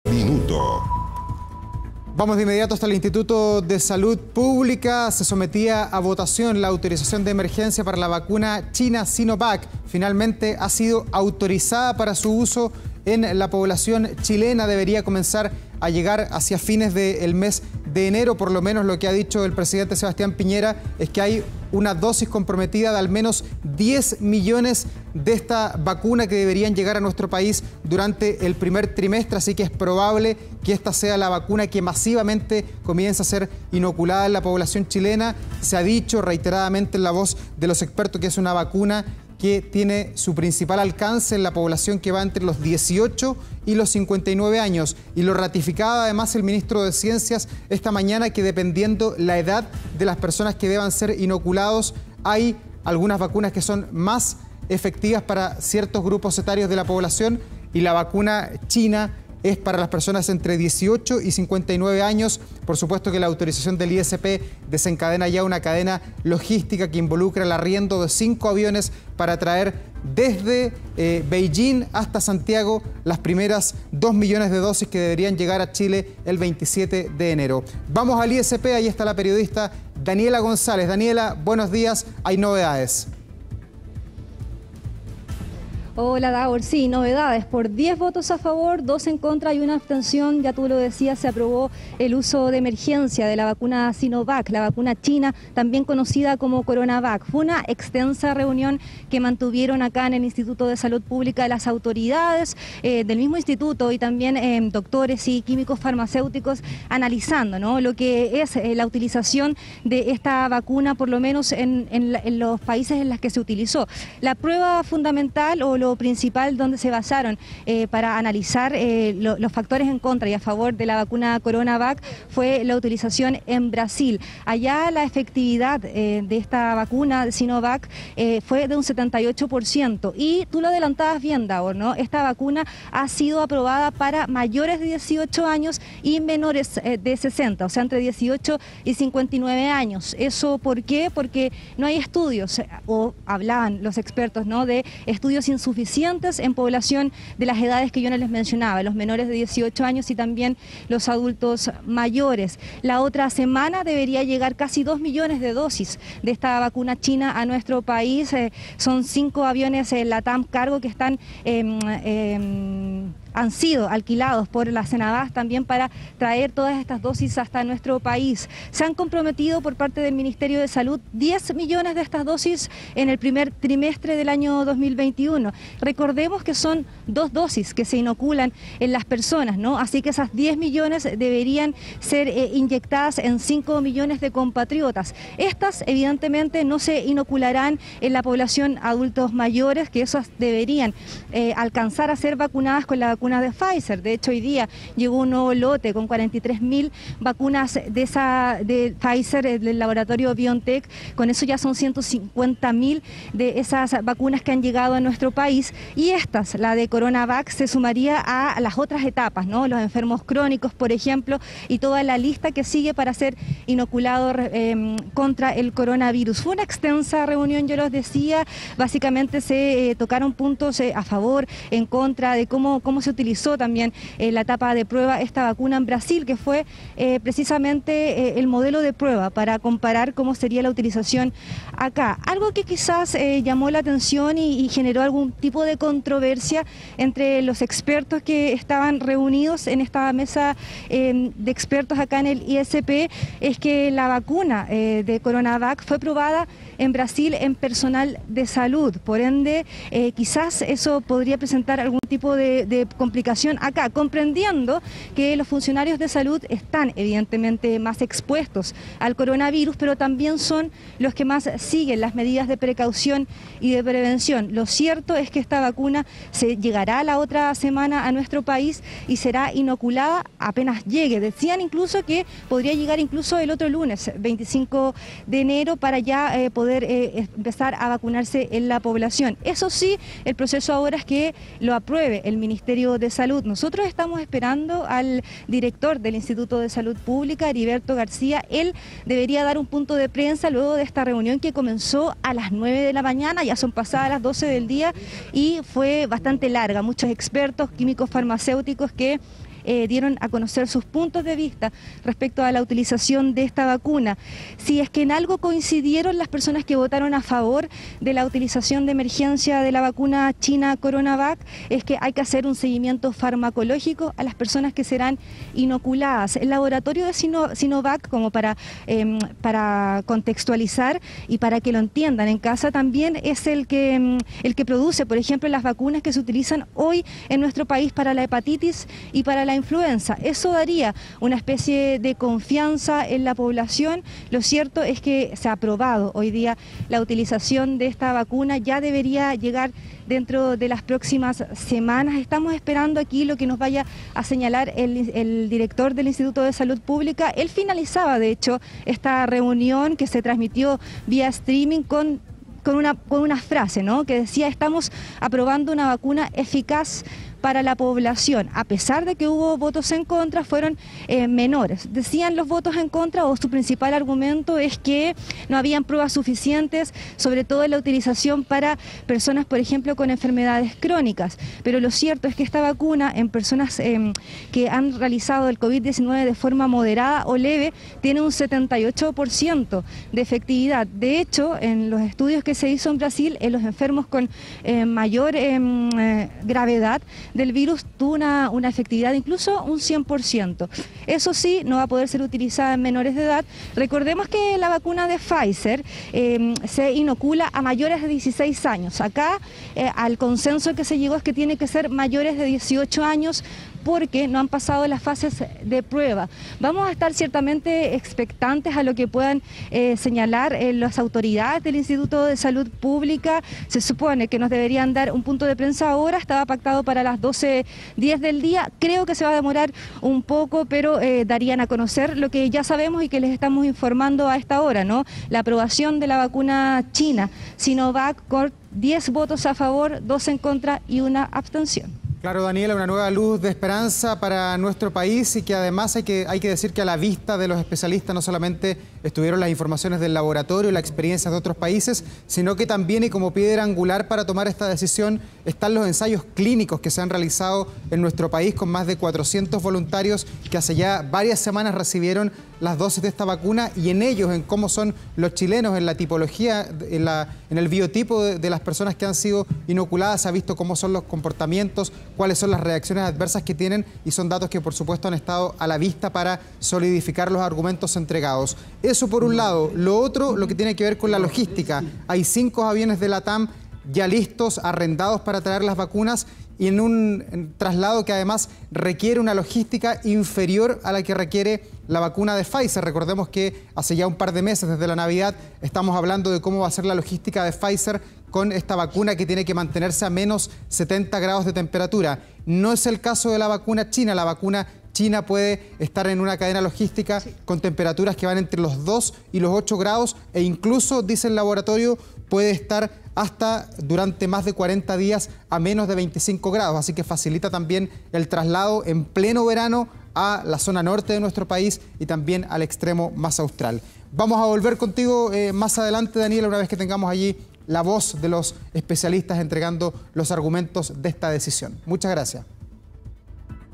ISP autoriza el uso de emergencia de la vacuna Sinovac
Con 10 votos a favor, dos en contra y una abstención, el Instituto de Salud Pública ha autorizado el uso de la vacuna china Sinovac en la población chilena entre 18 y 59 años.